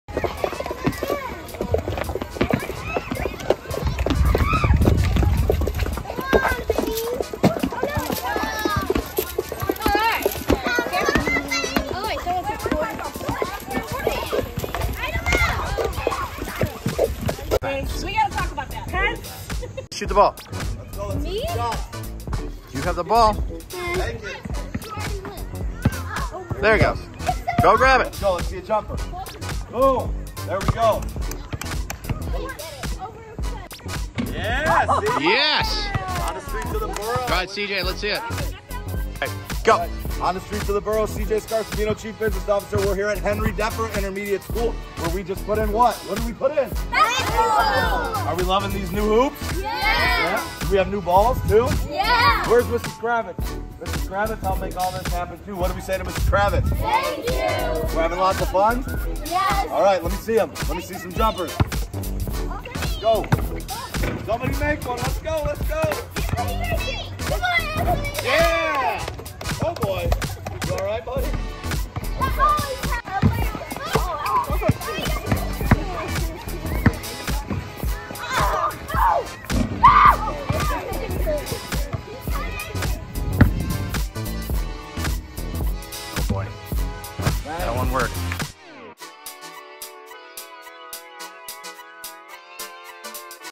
On the Streets of the BORO - HDI Gets New Hoops
Henry Defer Intermediate Schools gets a fresh set of hoops!